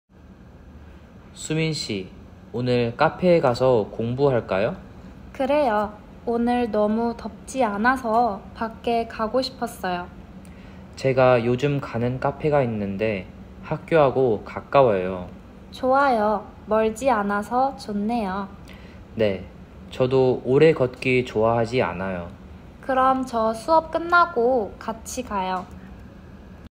(K102/K151) L16-5 V/A-s+지 않다 (1)
Example 1:

다니엘: 수민 씨, 오늘 카페에 가서 공부할까요?
수민: 그래요. 오늘 너무 덥지 않아서 밖에 가고 싶었어요.
다니엘: 제가 요즘 가는 카페가 있는데, 학교하고 가까워요.
수민: 좋아요. 멀지 않아서 좋네요.
다니엘: 네. 저도 오래 걷기 좋아하지 않아요.
수민: 그럼 저 수업 끝나고 같이 가요.

Example 1 (with English):

 Soomin, shall we go to a cafe to study today?
 Yes, that sounds good. I wanted to go outside because it's not too hot today.
 I know a cafe that I've been going to lately, and it's close to the school.
 That's great. It's nice that it's not far.
 Yes, I'm not a fan of walking long distances.
 Okay, let's go together after my class ends.